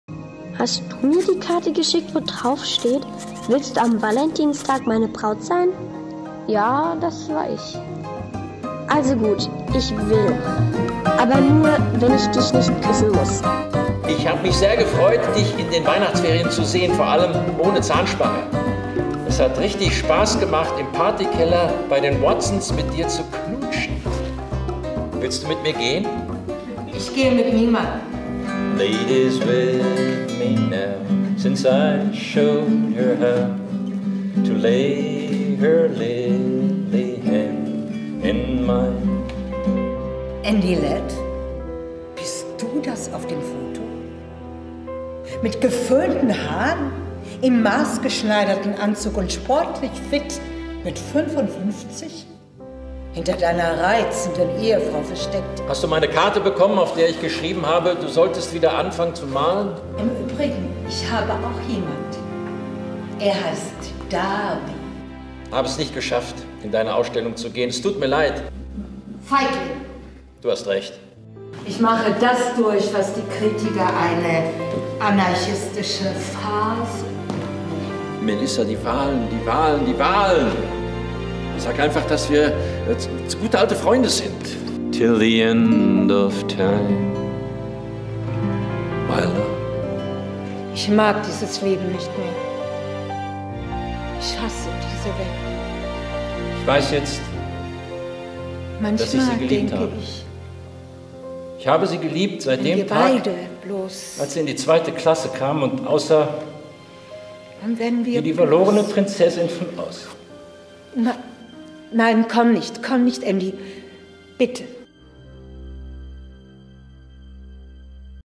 Love Letters
Love letters - Eine Frau, ein Mann und ihre Briefe.
Auf außergewöhnliche Weise wird der Zuschauer Zeuge eines lebenslangen Briefwechsels, der weit mehr ist als nur eine Brieffreundschaft.
Love letters ist die Geschichte einer großen Liebe. Skizzen zweier Leben, per Brief verbunden in guten und in schlechten Tagen – witzig, launisch, geistreich, faszinierend.
Ihre Wege trennen und kreuzen sich immer wieder, das Hoffen auf den anderen und seine Antworten aber hören nie auf.

Love Letters von A.R.Gurney

Inszenierungen im THEATER ALTES HALLENBAD Friedberg sowie Gallus Theater Frankfurt
mit Ingeburg Amodé & Armin Hauser
Regie Veronika Brendel